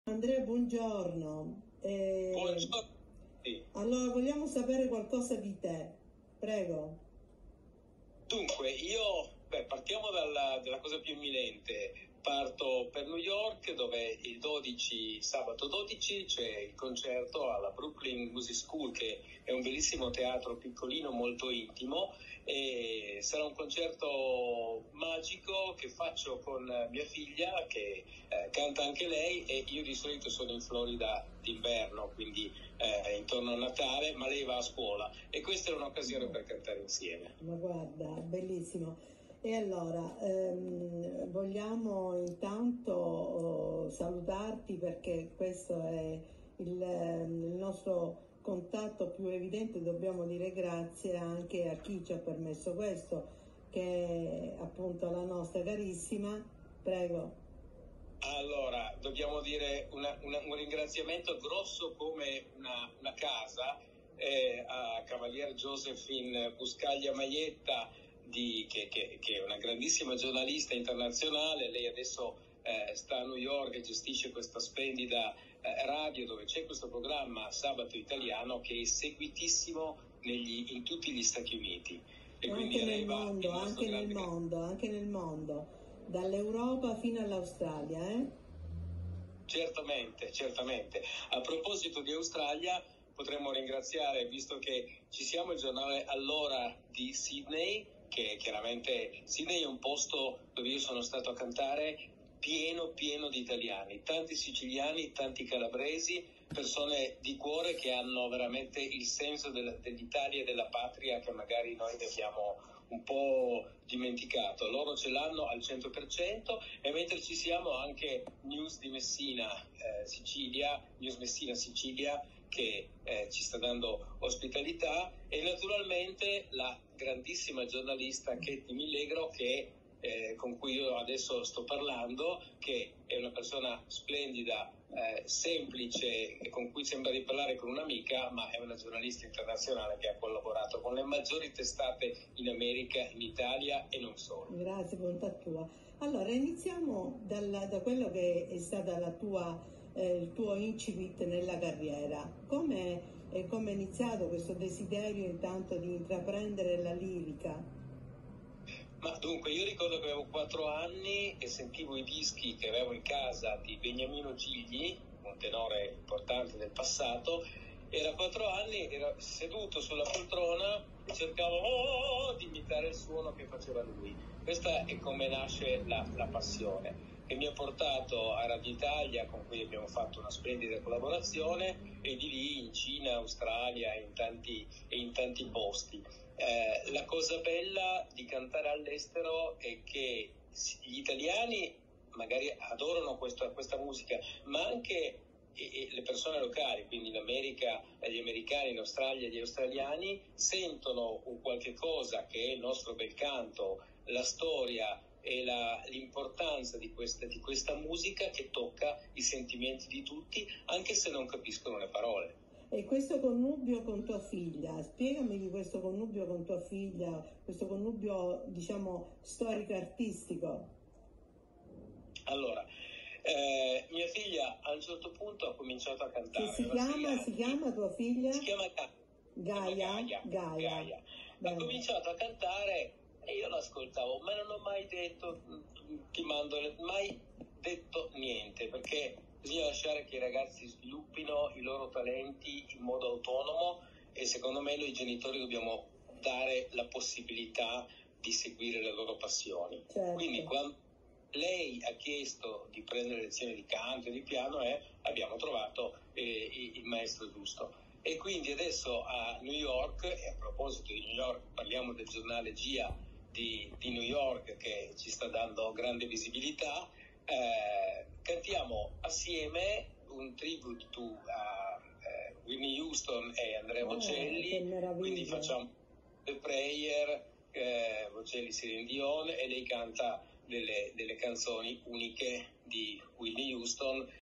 Andrea Del Principe
Andrea Del Principe da New York all’Australia per il Tributo ad Andrea Bocelli e Whitney Houston.